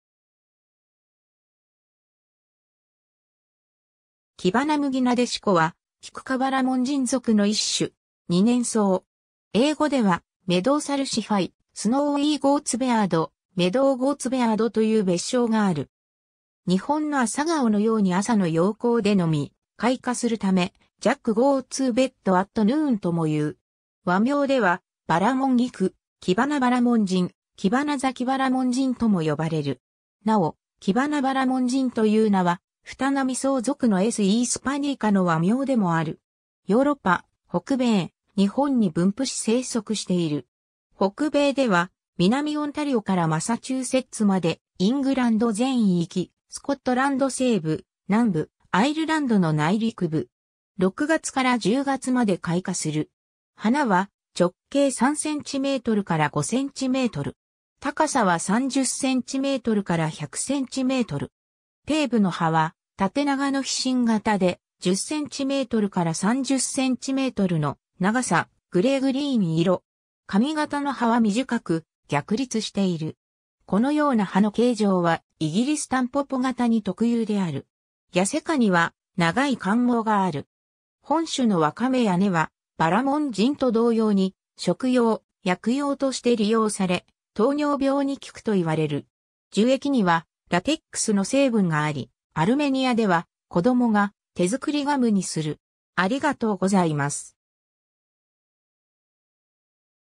キバナムギナデシコ
キバナムギナデシコ(学名:Tragopogon pratensis)は、キク科バラモンジン属の一種。
二年草。
 英語ではメドウサルシファイ、スノーウィーゴーツベアード、メドウゴーツベアードという別称がある。
日本のアサガオのように朝の陽光でのみ、開花するため、Jack-go-to-bed-at-noonともいう。
 和名では、バラモンギク、キバナバラモンジン、キバナザキバラモンジンとも呼ばれる。
なお、キバナバラモンジンという名は、フタナミソウ属の S. hispanica の和名でもある。
 ヨーロッパ、北米、日本に分布し生息している。
北米では南オンタリオからマサチューセッツまで、イングランド全域、スコットランド西部・南部、アイルランドの内陸部（湾岸地帯では生息していない）。
 6月から10月まで開花する。
花は直径3cmから5cm。
高さは30cmから100cm。
底部の葉は縦長の披針形で、10cmから30cmの長さ、グレーグリーン色。
上方の葉は短く、逆立している。
このような葉の形状はイギリスタンポポ型に特有である。
痩果には長い冠毛がある。
 本種の若芽や根はバラモンジンと同様に食用・薬用として利用され、糖尿病に効くといわれる。
樹液にはラテックスの成分があり、アルメニアでは子供が手作りガムにする。